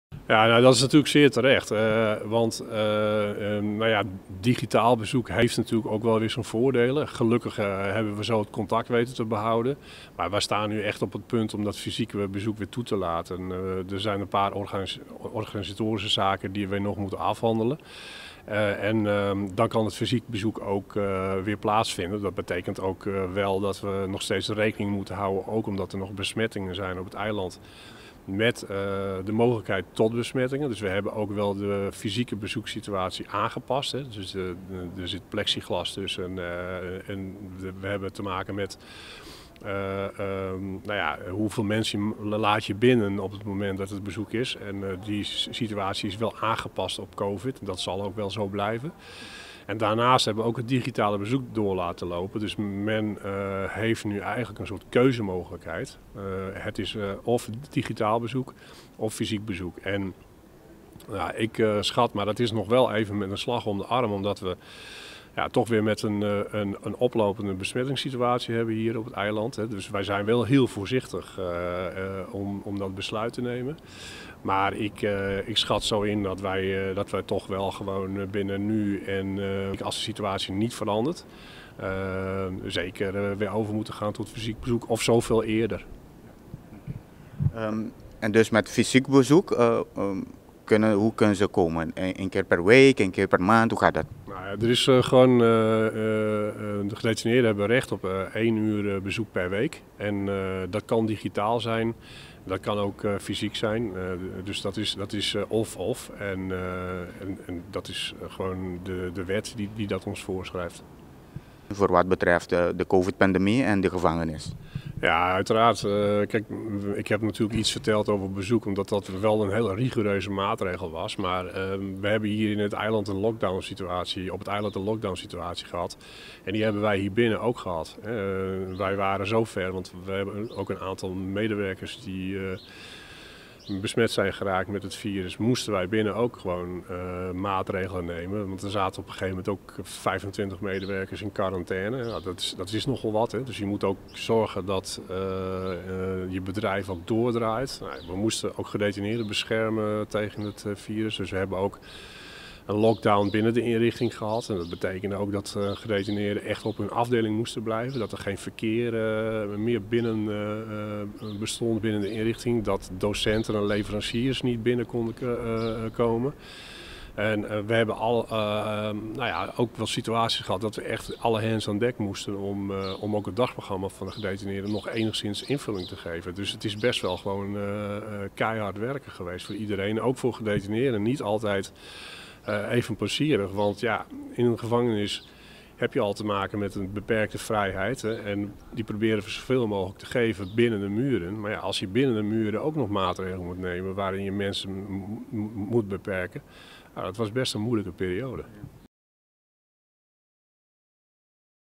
(2) Wibo de Vries Entrevista 2021
Reglanan ku mester a pone pa e detenidonan i trahadonan di e institushon a soru ku famianan tin mas di un aña i mei sin por tin kontakto personal ku nan famianan detene. Sr. de Vries ta amplia riba e parti aki